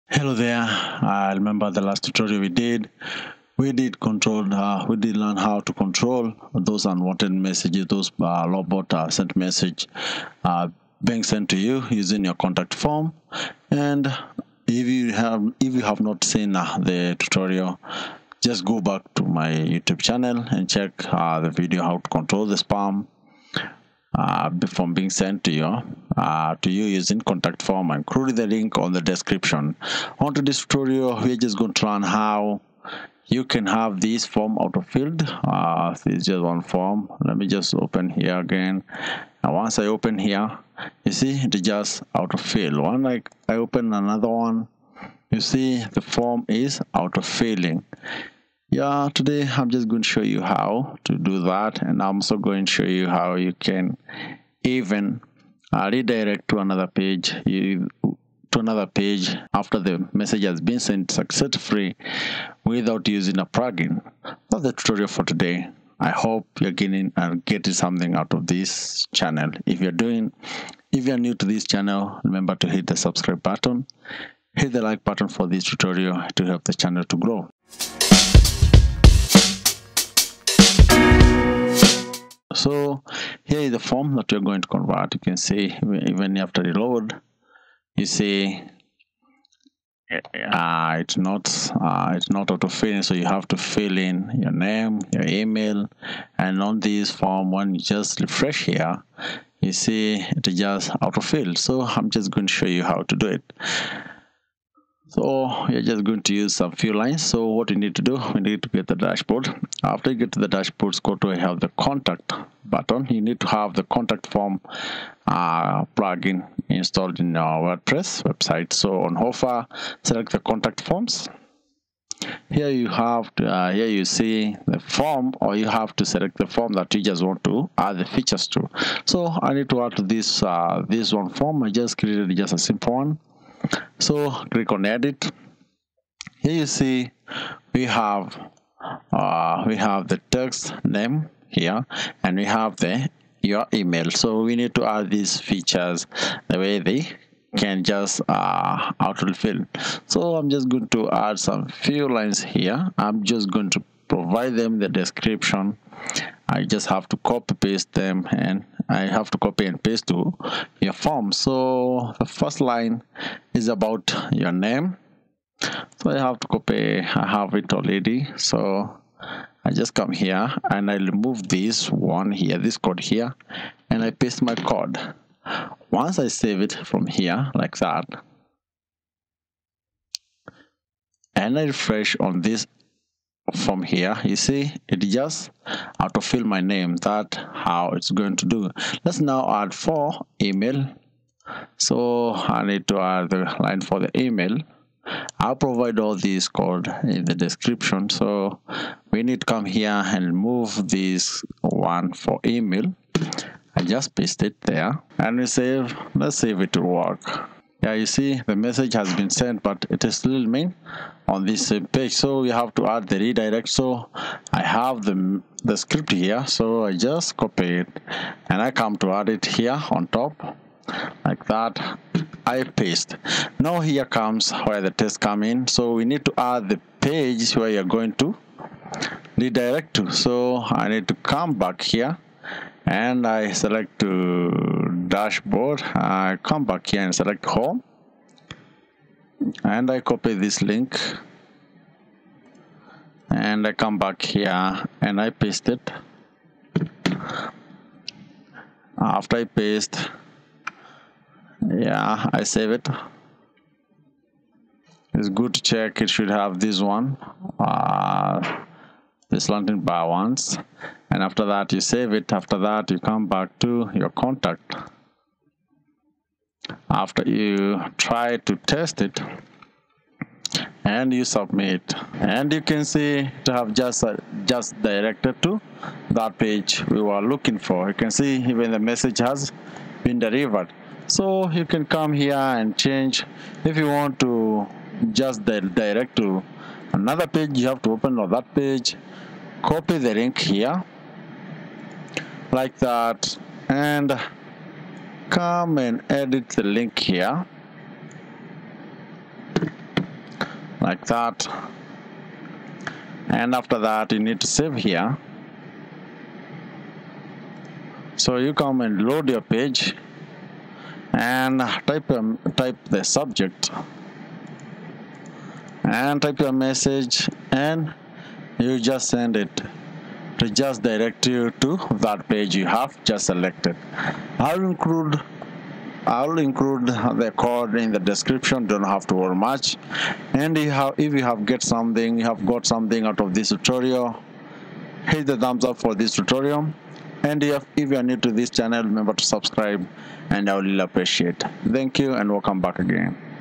contact form7 redirect and autofill without using plugin Solved in WordPress tutorial.
Learn how to solve contact form7 redirect and autofill without using a plugin. contact form7 will autofill and autofill automatic and after successful message sent, it just redirect to another page without a plugin in your WordPress website tutorial..

Short code needed:
Your Name: [text* your-name default:user_display_name]

Redirect to another page shortcode on comment section.

★Timestamp★
★★★ 00:00   Introduction,
★★★ 01:22   Get the dashboard
★★★ 02:53   Add name Autofill
★★★ 03:15   Add email autofill
★★★ 04:00    Add Redirect to another page